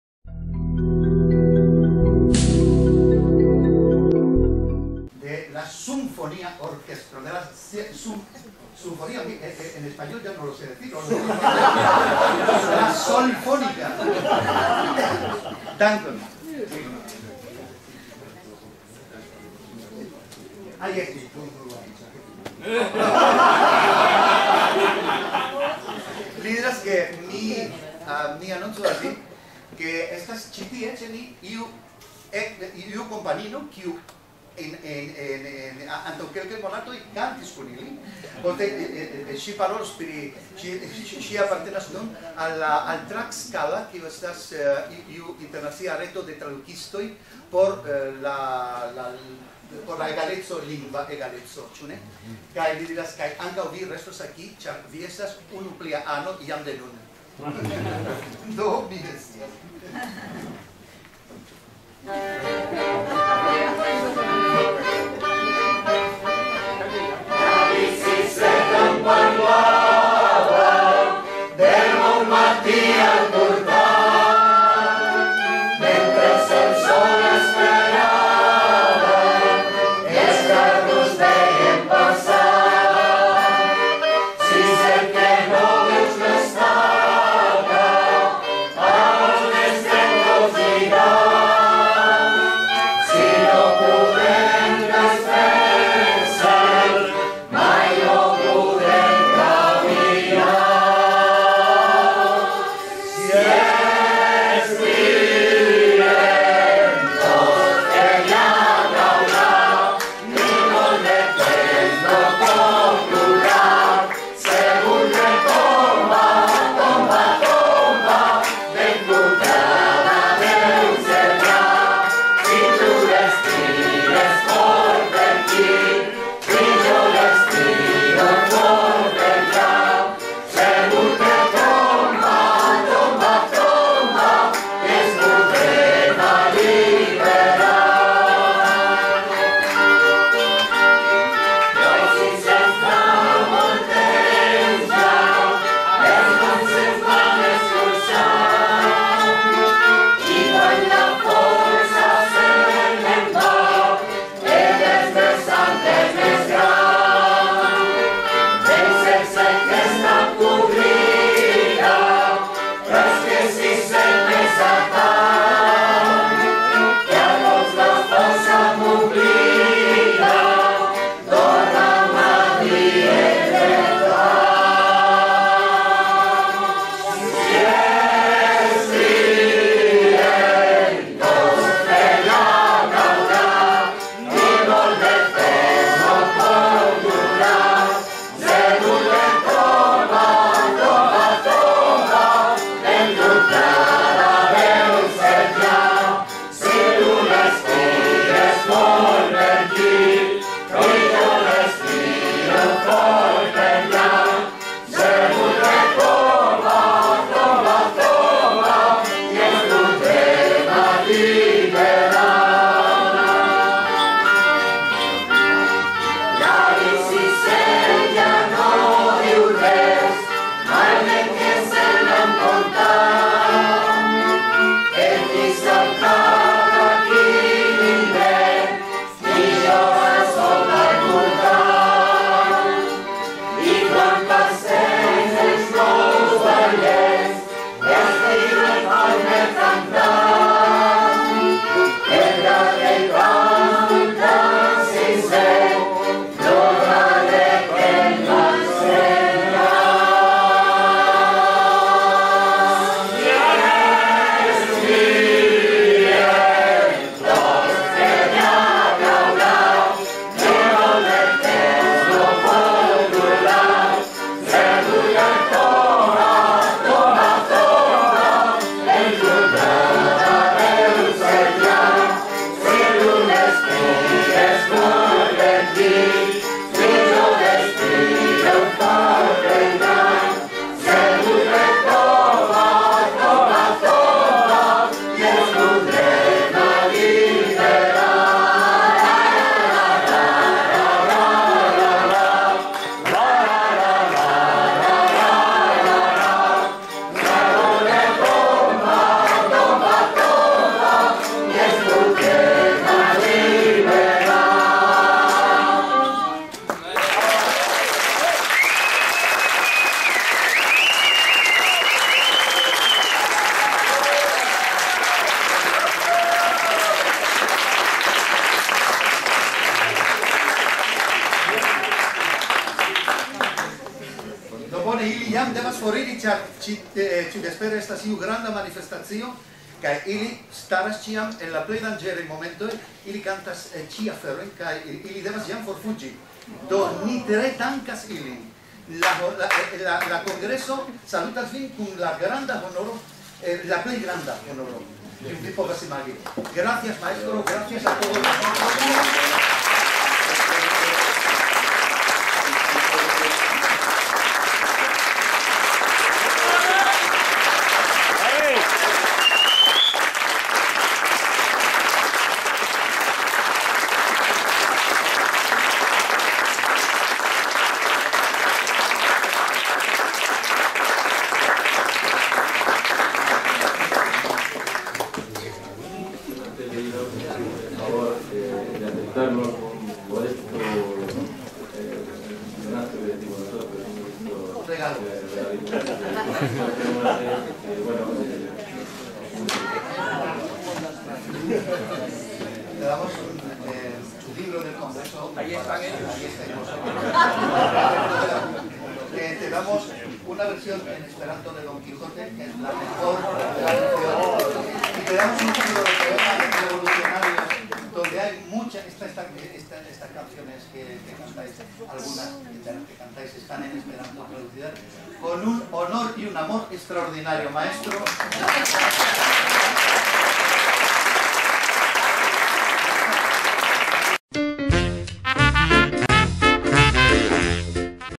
La Solfónica - La indignula movado kun Esperanto
"La Solfónica" estas koruso kaj orkestro, kiu ŝprucis el la indignula movado. Ĝi partoprenas nur agadojn kun depostula karaktero, kaj ni havis la fortunon ke ili rigardis tiuspeca la 86an SAT-Kongreson. Kaj parto de la orkestro vizitis nin!